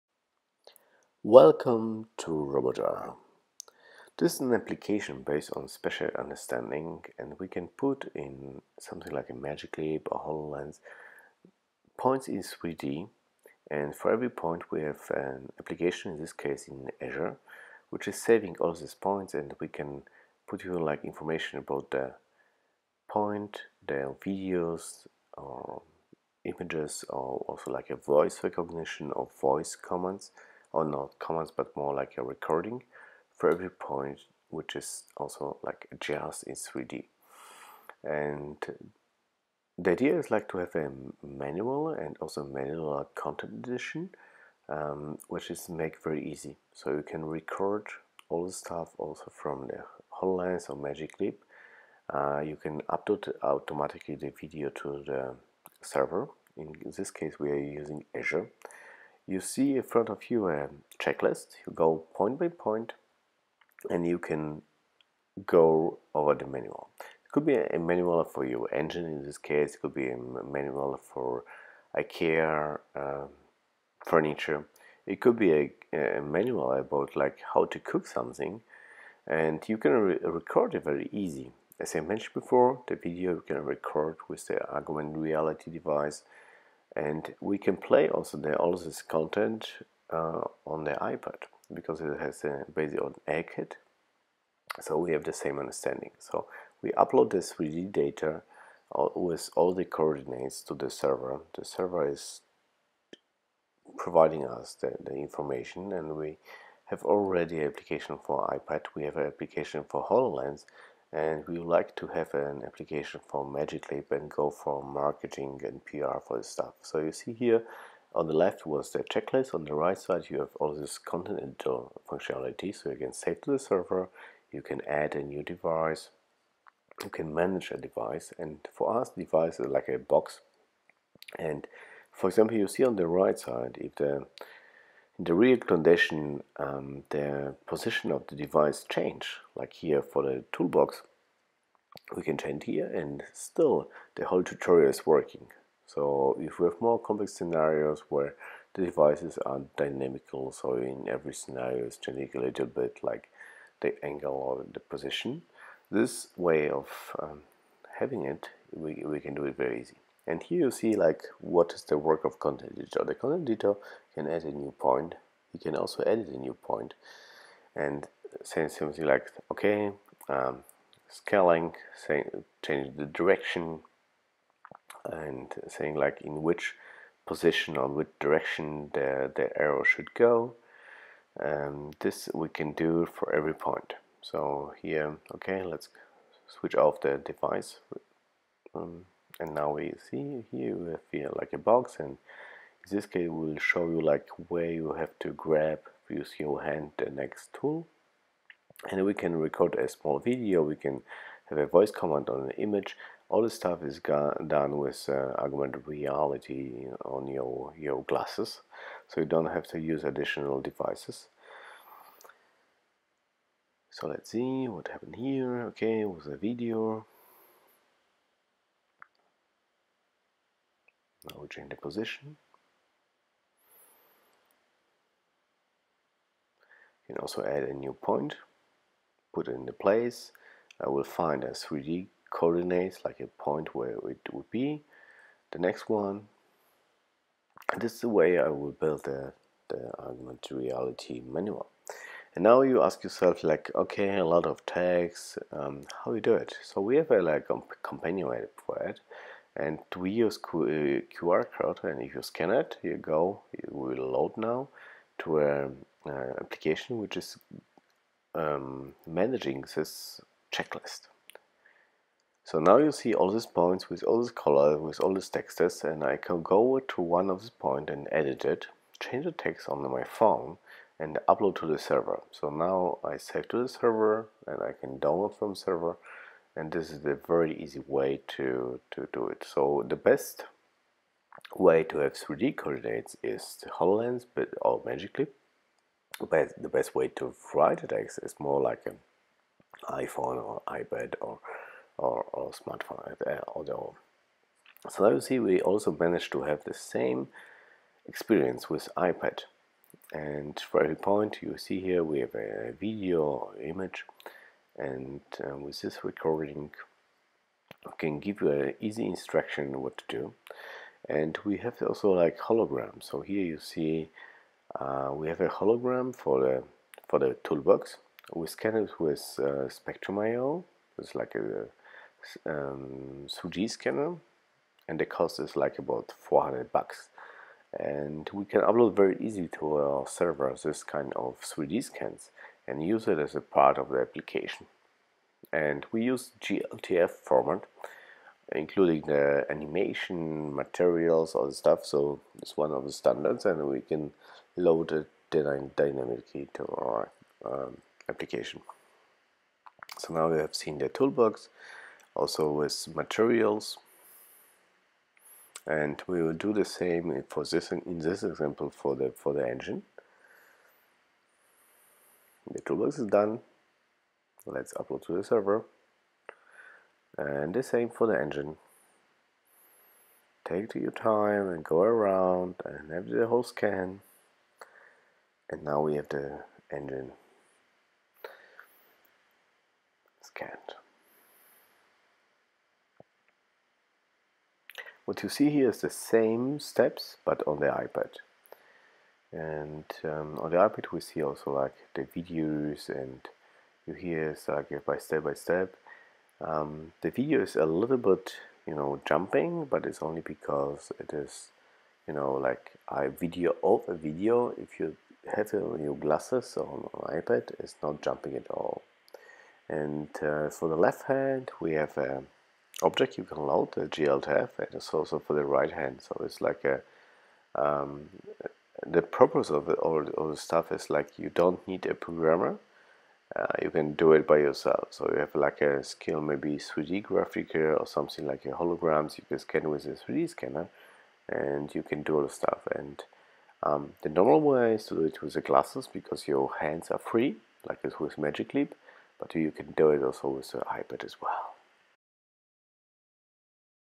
RobotAR for HoloLens, iPad and Magic Leap (next)
Easy tutorials, next generation, with devices like HoloLens or iPad ( AirKit ) and MagicLeap (next).
No code! Just create the tutorial.

We have been working on this project for longer, but now we reached a level, we would like to share with public.

Create own tutorials in minutes. Add content like video, pictures or voice recording. On a smartphone you can edit the text, color, change the pointer from an arrow to a left rotation etc.
The content is stored in the Azure Cloud but you can use Azure Stack for a OnPrem scenario.
The app is working on HoloLens and on iPad in the view mode. You can have a collaboration for education as well, connecting multiple devices in one online session.
Use it for service worker, to help them with scenarios, they are not executing every day.
Or prepare the cooking instructions for your family;)